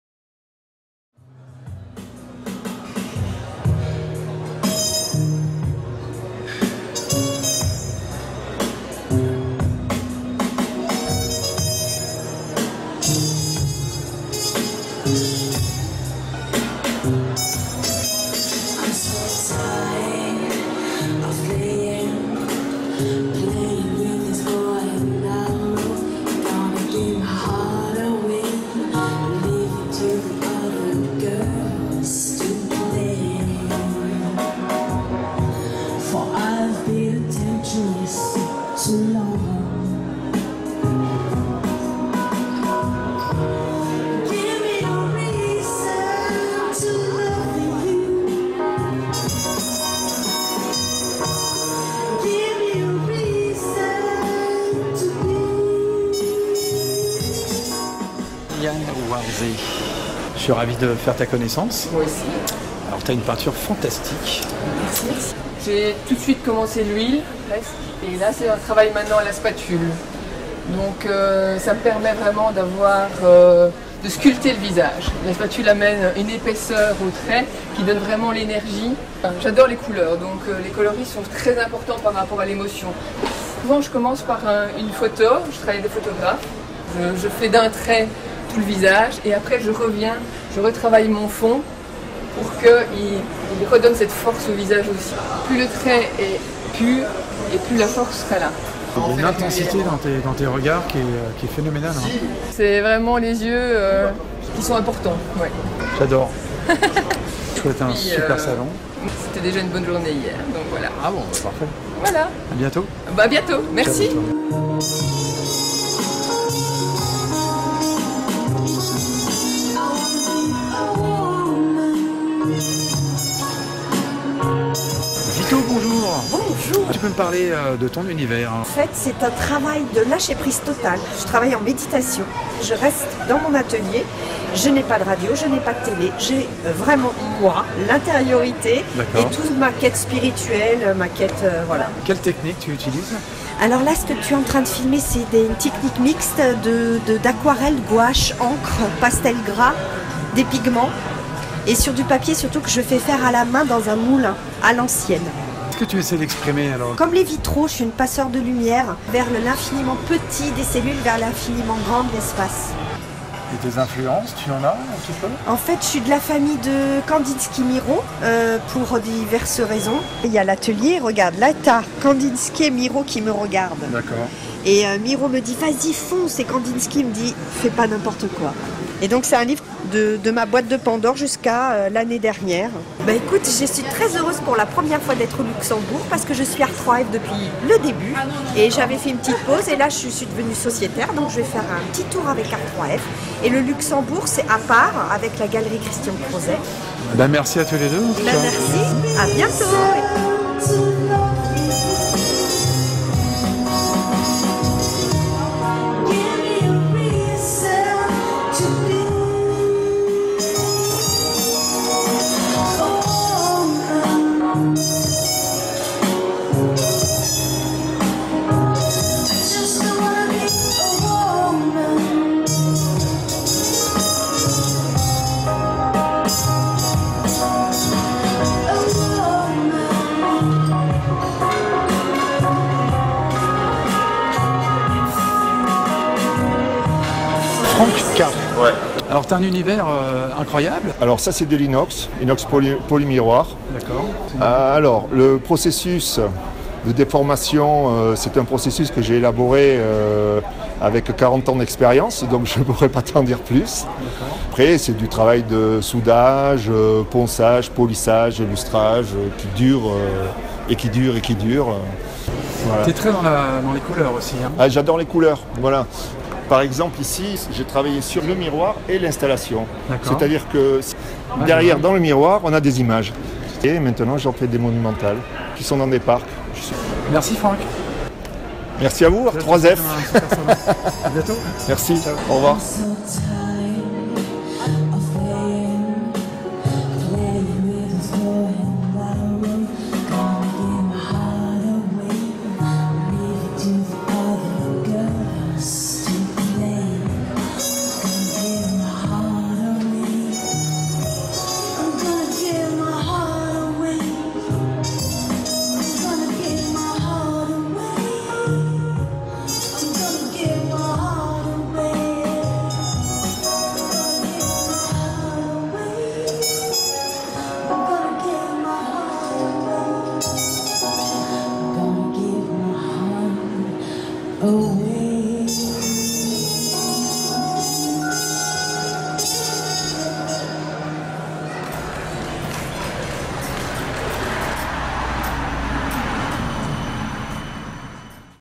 Art3F Luxembourg - Juin 2018
Art3F Luxembourg - Juin 2018 - Merci aux Artistes et à toute l'équipe d'Organisation - Reportage @ David Law - Facebook.com/ParisPhotoArt - Musique Live Amanova - Prochain RDV Art3F à Cannes au Palais des Festivals du 27 au 29 Juillet 2018.